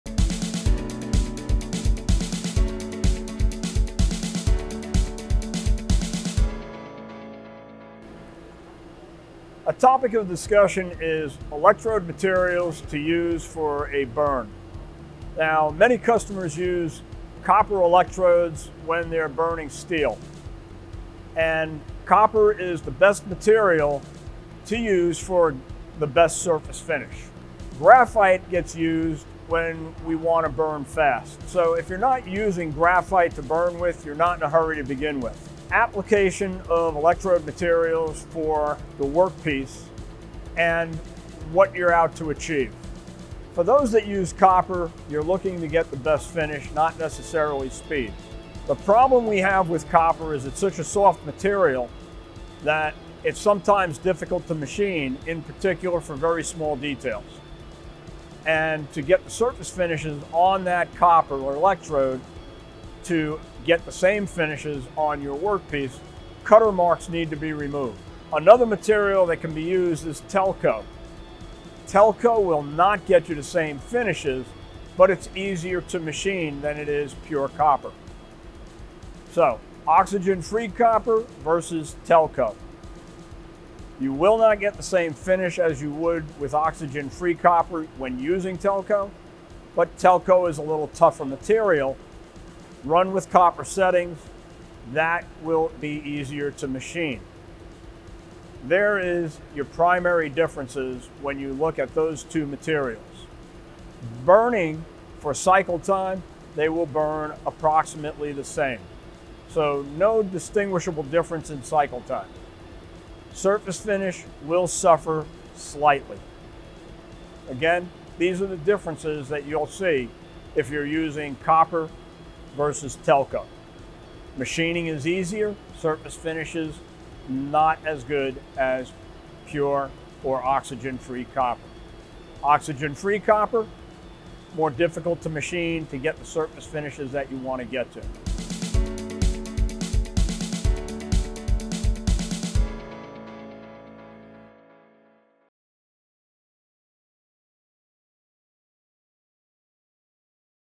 Electrode Materials for Sinker EDMs Copper vs Graphite
Join one of Makino's EDM Application Engineers for best practice advice on Sinker EDM electrode material choices. In this Makino Tech Tip, we discuss the characteristics, advantages and disadvantages of copper compared to graphite.